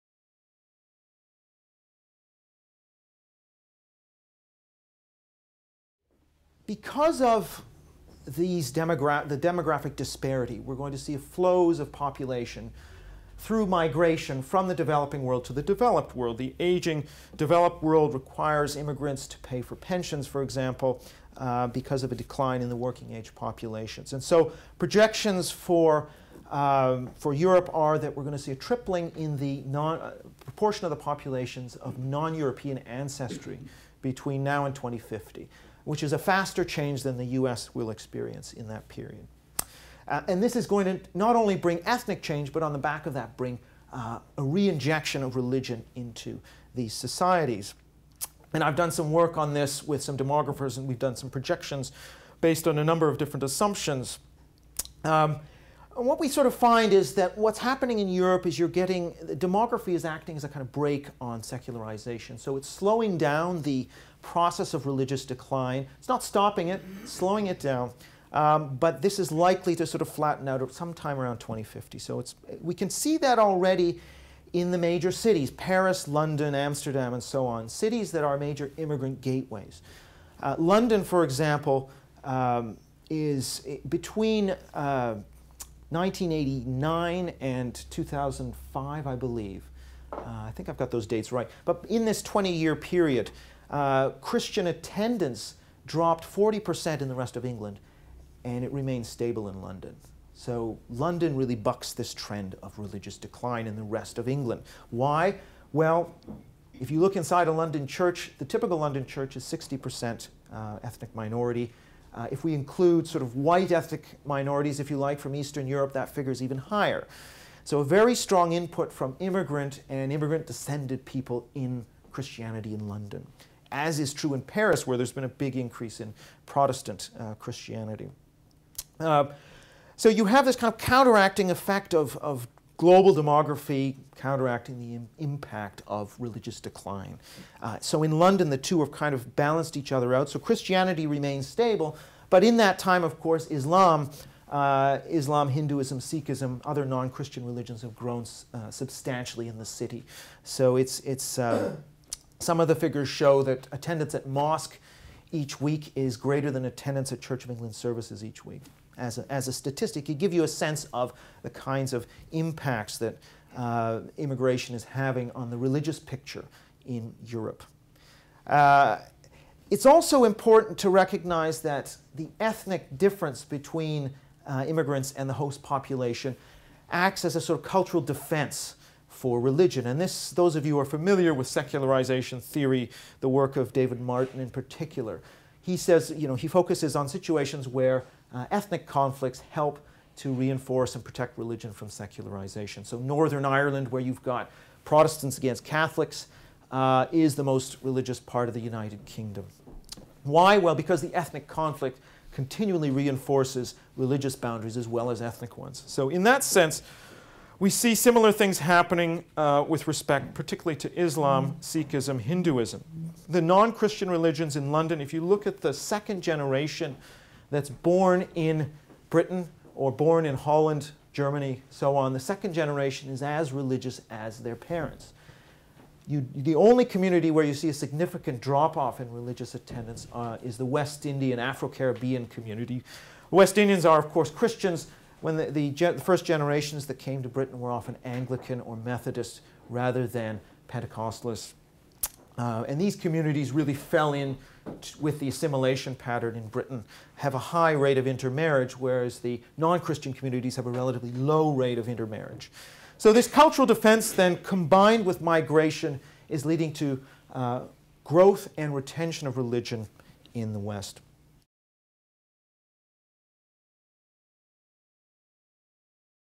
Eric Kaufmann on the Impact of Immigration on Religious Picture in Europe
January 11, 2012 | This video has been excerpted from the Berkley Center's event Shall the Religious Inherit the Earth? Demography and Politics in the Twenty-First Century.

Dr. Eric Kaufmann discussed his new book Shall the Religious Inherit the Earth: Demography and Politics in the Twenty-First Century. Based on demographic research focused on immigration, multiculturalism, and the inheritance of religious belief, the author explored the effects of the rise of religious conservatism and the corresponding decline in liberal secularism.